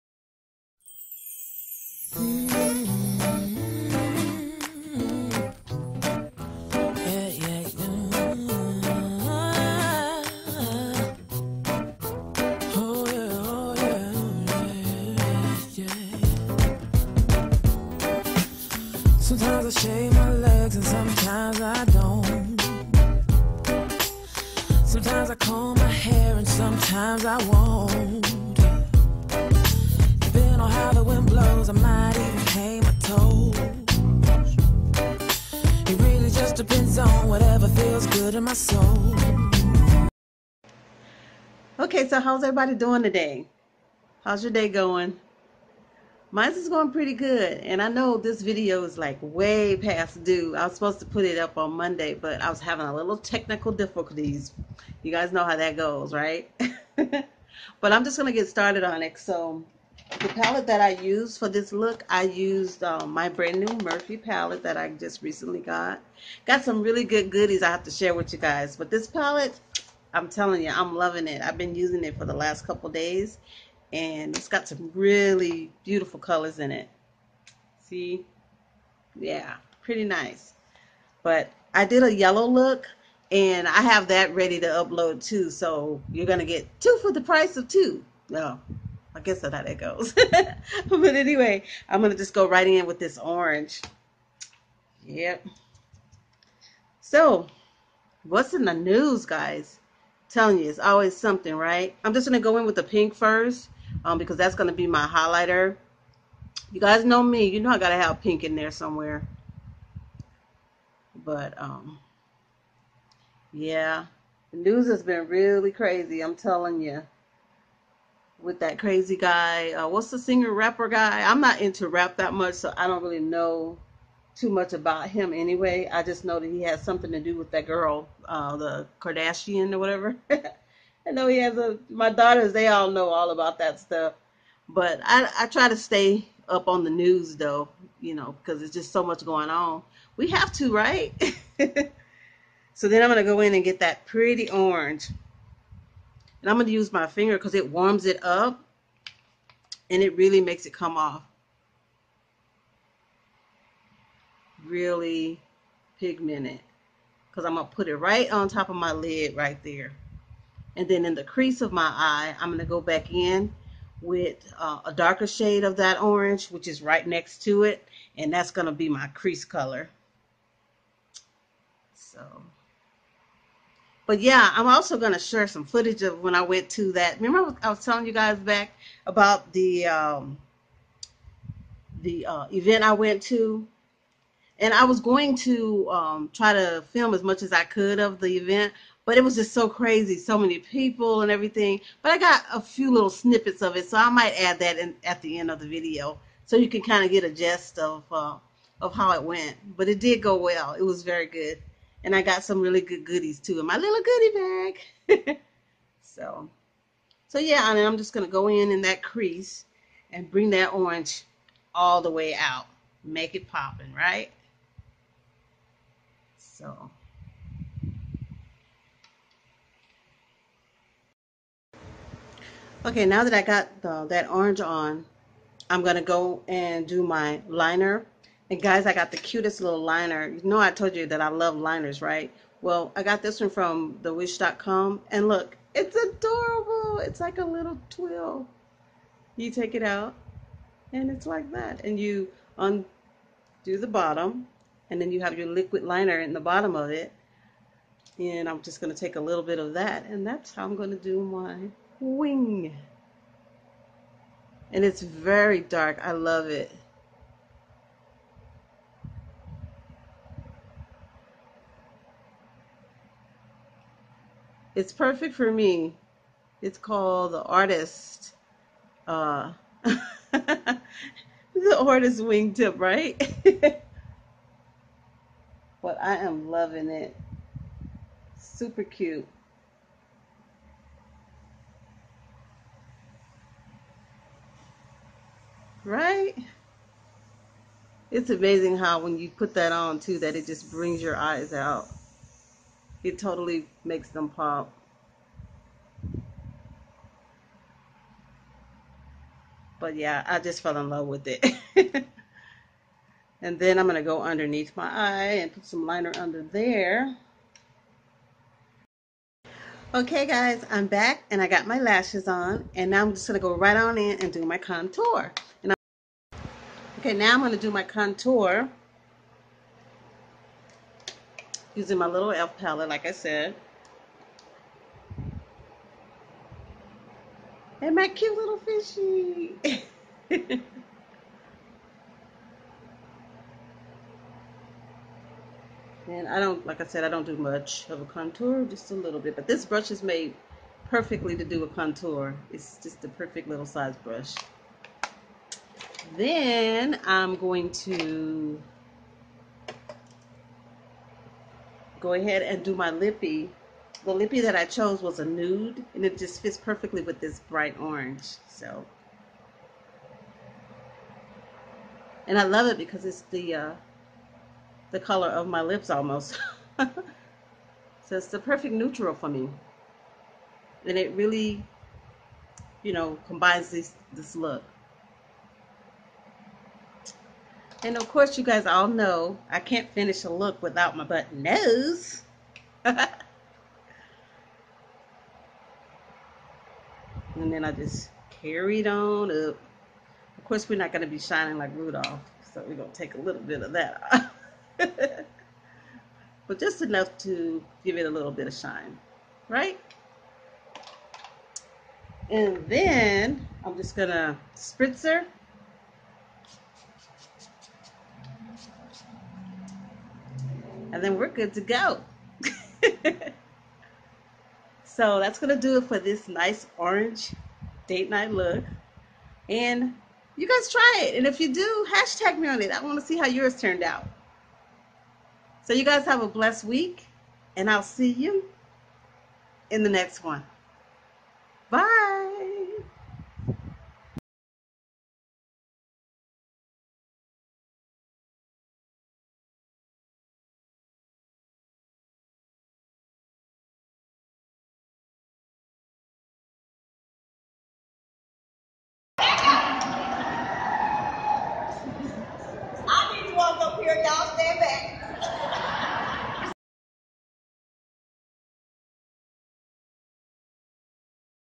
Monday Makeup: ORANGE is the color for fun datenite
WELCOME! to my channel for another exciting video of me sharing with you my datenite looks...this time i am wearing orange...i did it quick, simple and easy...i also will show a couple pics of my experience at the health expo..i attend it yearly with my daughter,,it was once again a success...so sit a while 7 mins to be excate and enjoy like and when you do SUBscribe!!